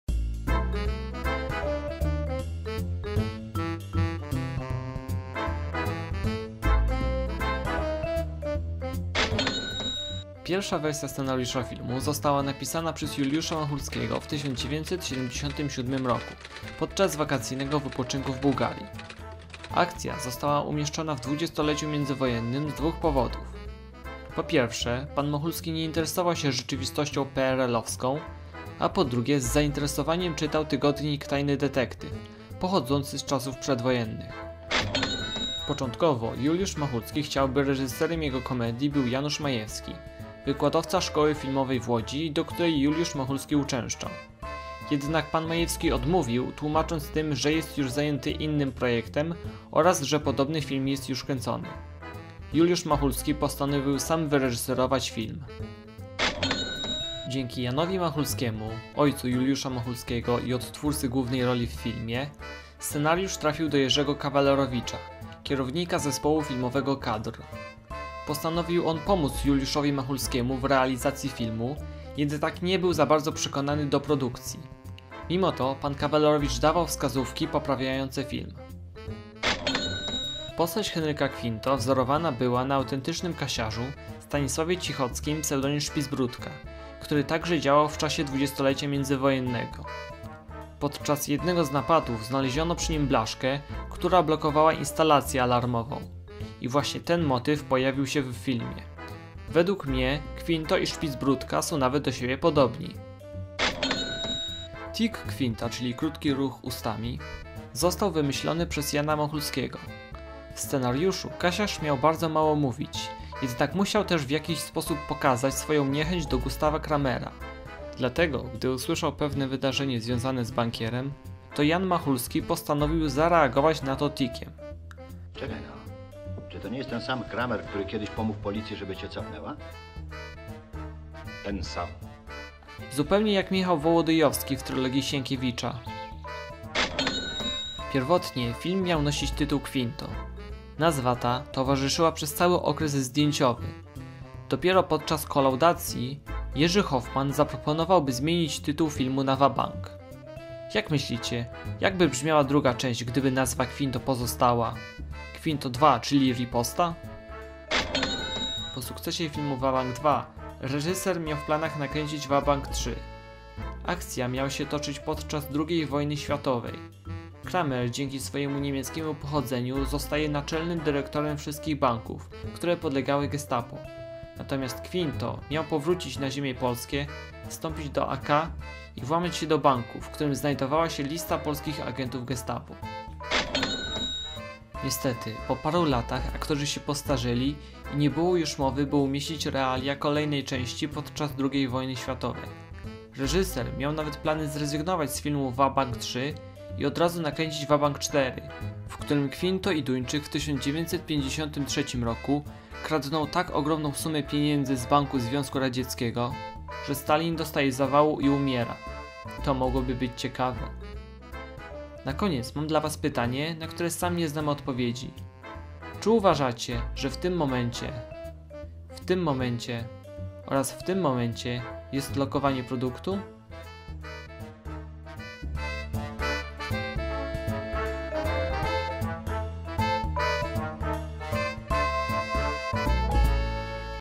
8 ciekawostek z filmu "Vabank" | Poznać kino
Debiutancki film Juliusz Machulskiego pt. "Vabank" jest jedną z najbardziej znanych polskich komedii. Historia o oszukanym kasiarzu, Henryku Kwinto, zachwyciła widzów, Poznajcie 8 ciekawostek z filmu "Vabank"

Źródło:
magazyn "Kino" (nr 10/2011) - wywiad z Juliuszem Machulskim . - "VABANK, Jan i Juliusz Machulscy, Warszawa 2007" - "Akademia Polskiego Filmu"
Radio Dwójka - wywiad z Juliuszem Machulskim.